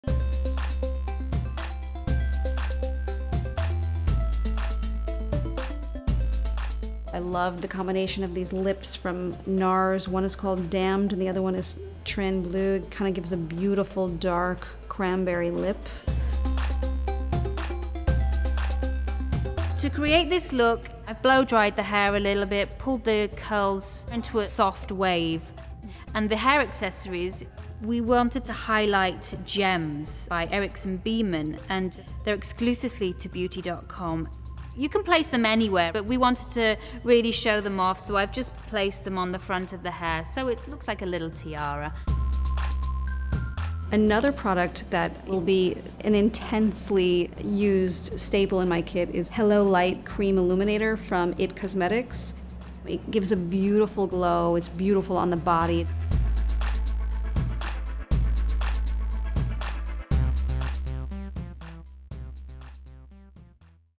Holiday 2013 Trends: GEMS for GEMS
Part of the Holiday 2013 trends, exclusively on Beauty.com.

Makeup:
Romy combined two of the NARS Lip Pencil -- Damned and Train Bleu -- to create a dark, cranberry lip color. She then uses IT Cosmetics Hello Light Crème Illuminator, which is an intensely used stable in her kit, to highlight and create a beautiful glow on the body.

Hair:
Esther Langham blow dried the hair and pulled the curls back into a soft wave. She finished off the look with Gems for GEMS hair accessories by Erickson Beamon for Beauty.com. To show them off like a little tiara, she placed them in front of the model's hair.